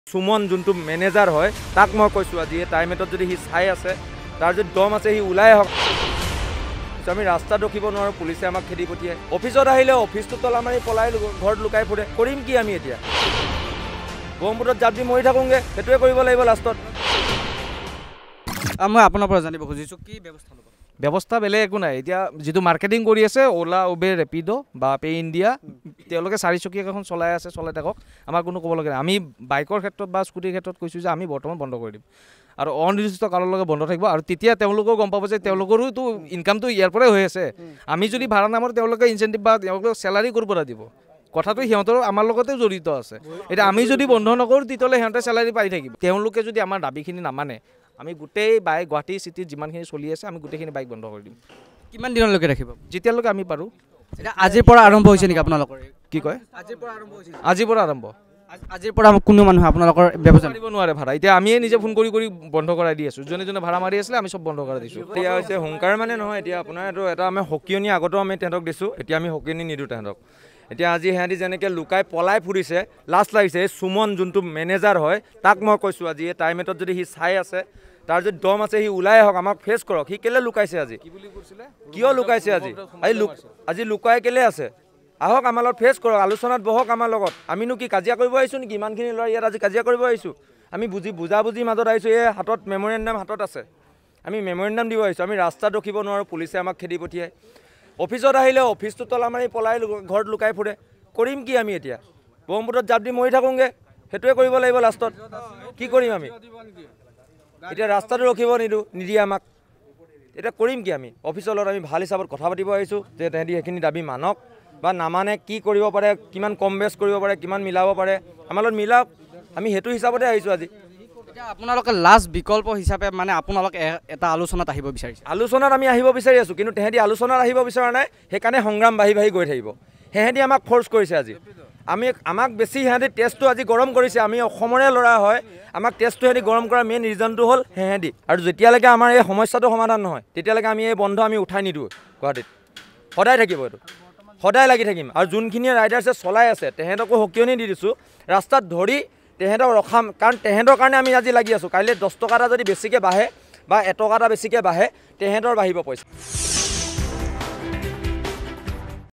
অলা,উবেৰ,ৰেপিড'ৰ প্ৰতি বিক্ষোভ কিয় কৰ্মচাৰীৰ?
'আমি ৰাস্তাত ৰাখিব নোৱাৰোঁ পুলিচে খেদে , অফিচলৈ আহিলে তলা মাৰি দিয়ে , কৰিম কি এতিয়া আমি? ব্ৰহ্মপুত্ৰত জাপ দি মৰি থাকোঁ সেইটোৱে কৰিব লাগিব শেষত । অলা,উবেৰ,ৰেপিড'ৰ প্ৰতি এই বিক্ষোভ কিয় কৰ্মচাৰীৰ?